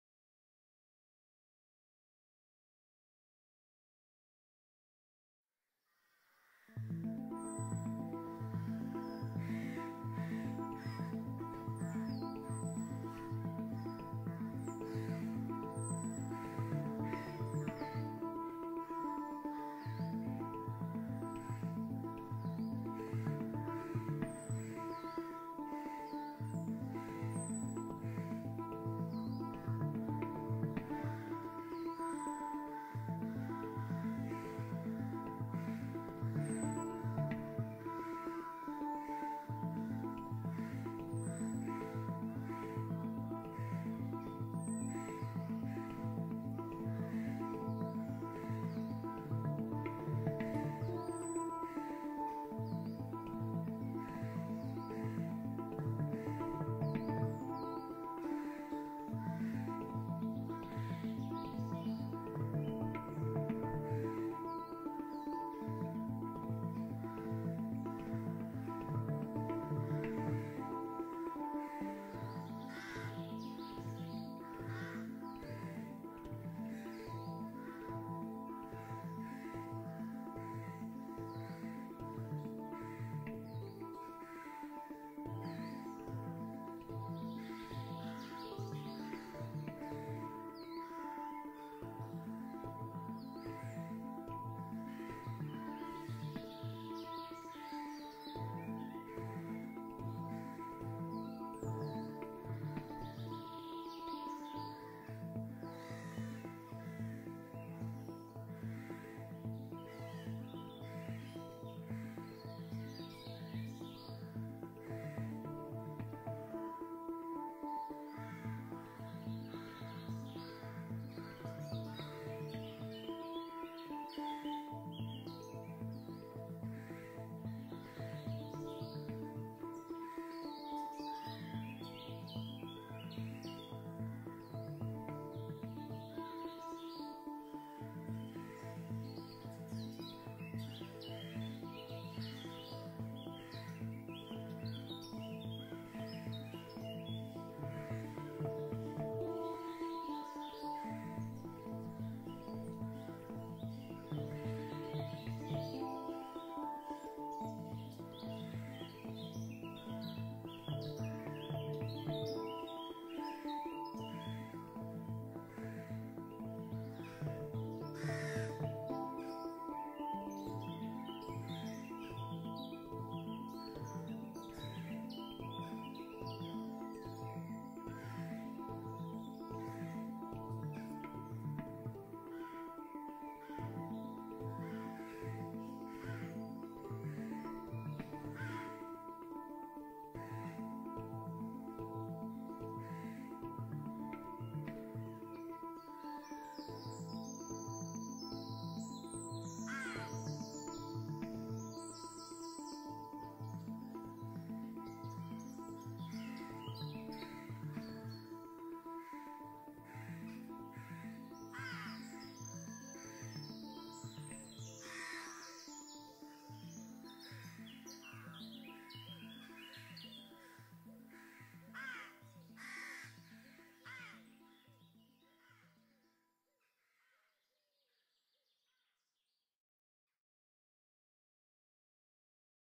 The Tattie Field
Slo Tee Vee. Filmed near home with the micro Osmo Pocket. Music written in Logic x

The birds. were recorded in a wood nearby.